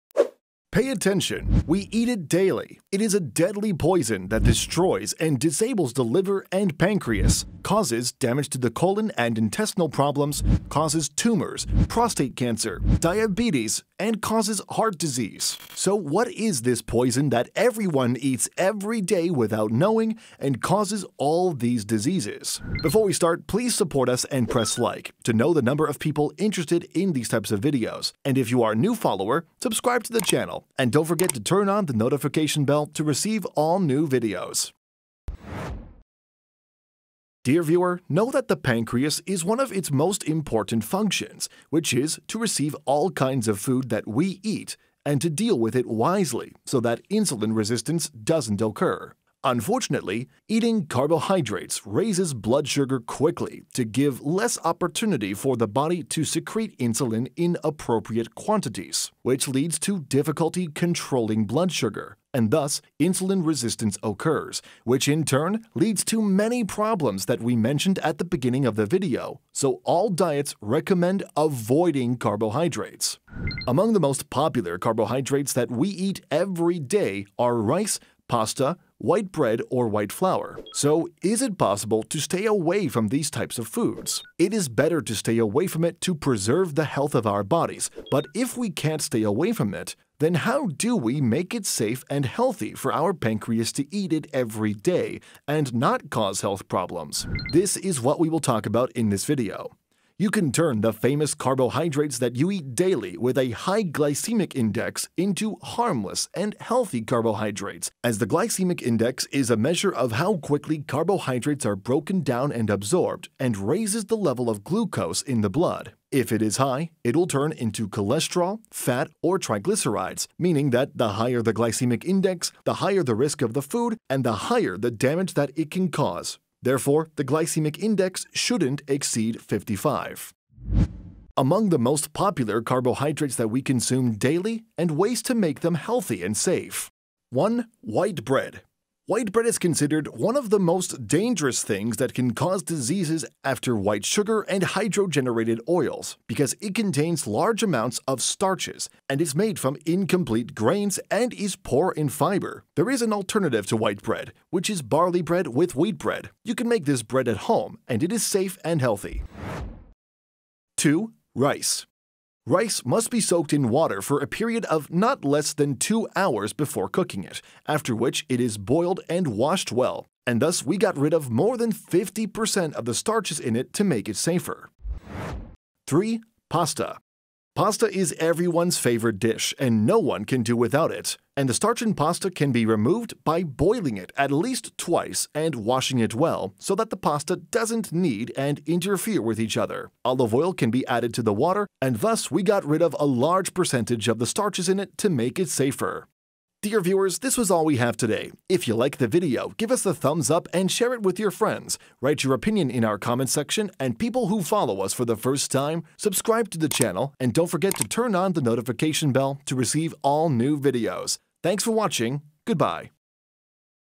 Please Pay Attention to This disaster,it Destroys the Pancreas,Damages Cells,&Ruins The body &Organs
Carbohydrates are one of the most common types of food that we eat continuously, but unfortunately, consuming carbohydrates irregularly leads to many serious problems within our bodies. In this video we will explain the dangers of consuming carbohydrates, in addition to if we cannot avoid eating carbohydrates, how do we make them safe and healthy for our pancreas?, And without causing health problems.